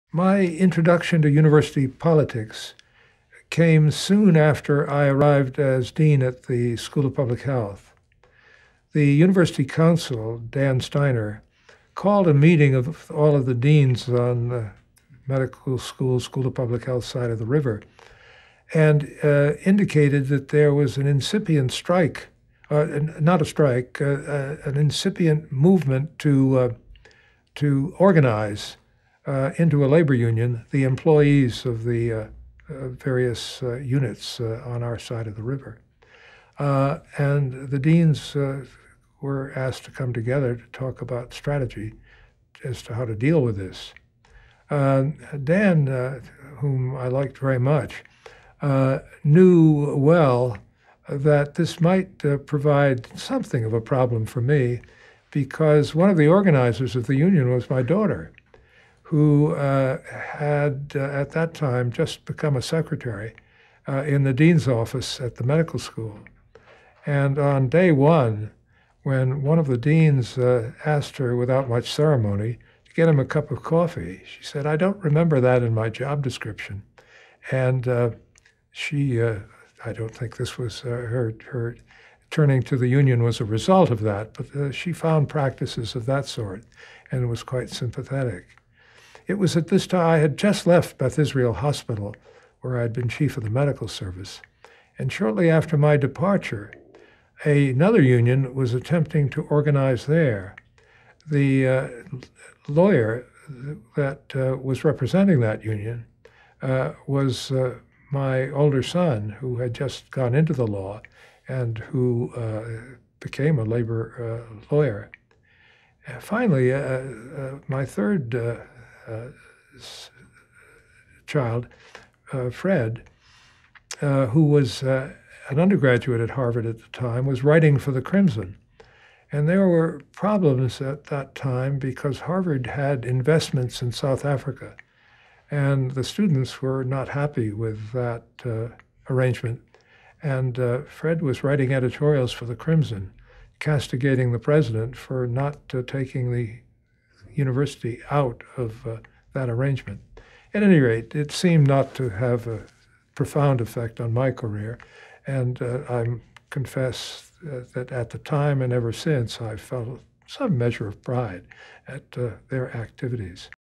Howard Hiatt - My politicized children (26/63)
Born in America in 1925, Howard Hiatt set up one of the first medical oncology research and training units in the US and has led some of most renowned US medical institutions. He is also committed to helping disadvantaged people access good health care. [Listener: Milton C. Weinstein; date recorded: 2006]

TRANSCRIPT: My introduction to university politics came soon after I arrived as dean at the School of Public Health. The university counsel, Dan Steiner, called a meeting of all of the deans on medical school... School of Public Health side of the river, and indicated that there was an incipient strike, not a strike, an incipient movement to... to organize into a labor union the employees of the various units on our side of the river, and the deans were asked to come together to talk about strategy as to how to deal with this. Dan, whom I liked very much, knew well that this might provide something of a problem for me because one of the organizers of the union was my daughter, who had at that time just become a secretary in the dean's office at the medical school, and on day one, when one of the deans asked her without much ceremony to get him a cup of coffee, she said, 'I don't remember that in my job description'. And, she... I don't think this was her... her turning to the union was a result of that, but she found practices of that sort and it was quite sympathetic.

It was at this time - I had just left Beth Israel Hospital where I'd been Chief of the Medical Service - and shortly after my departure another union was attempting to organize there. The lawyer that was representing that union was my older son who had just gone into the law and who became a labor lawyer. Finally, my third child, Fred, who was an undergraduate at Harvard at the time, was writing for 'The Crimson', and there were problems at that time because Harvard had investments in South Africa and the students were not happy with that arrangement and Fred was writing editorials for 'The Crimson', castigating the President for not taking the university out of that arrangement. At any rate, it seemed not to have a profound affect on my career and I confess that, at the time and ever since, I've felt some measure of pride at their activities.